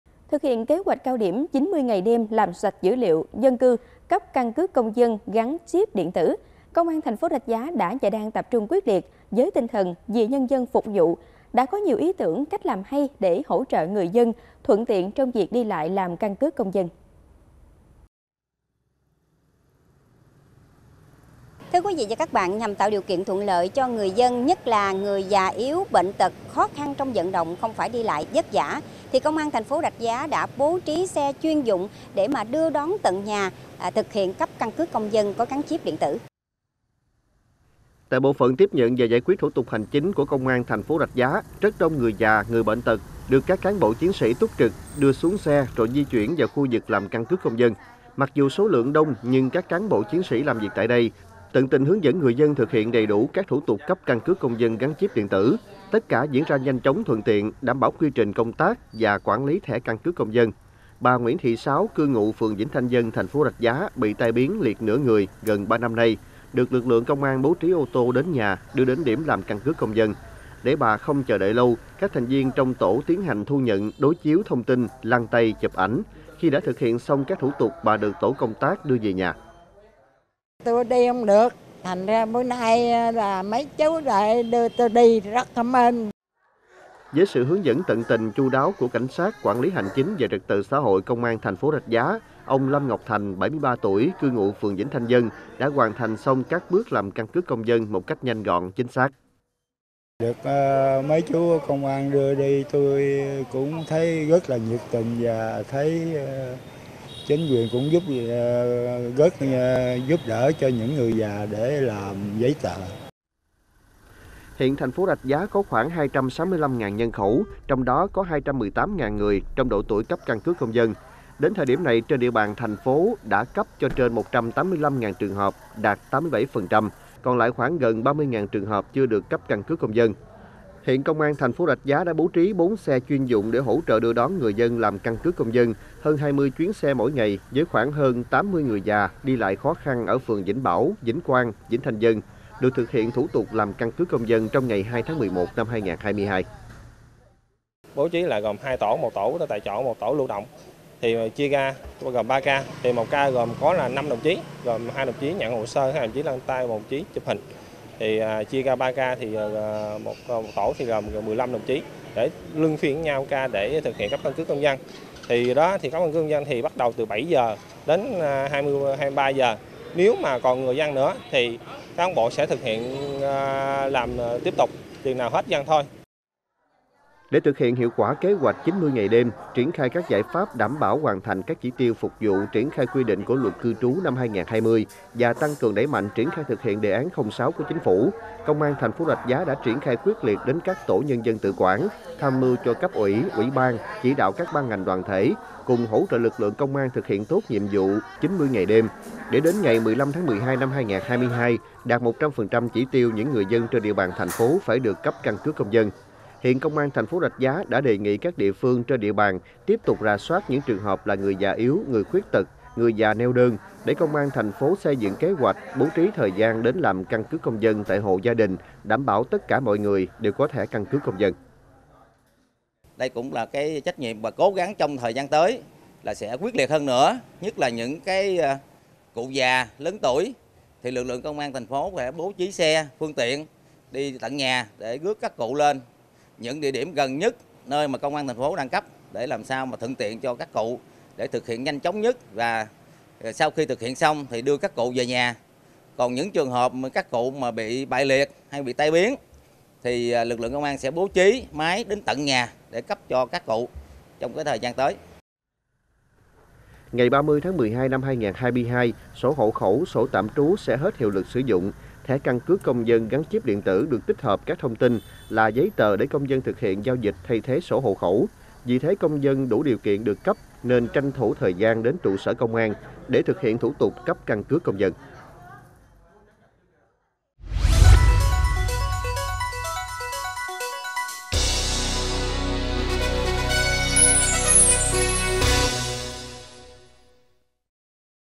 Những chuyến xe vì dân | THKG
Thực hiện kế hoạch cao điểm “90 ngày, đêm” làm sạch dữ liệu dân cư, cấp căn cước công dân (CCCD) gắn chíp điện tử, Công an thành phố Rạch Giá đã và đang tập trung quyết liệt.  Với tinh thần “Vì Nhân dân phục vụ" đã có nhiều ý tưởng, cách làm hay để hỗ trợ người dân thuận tiện trong việc đi lại làm CCCD.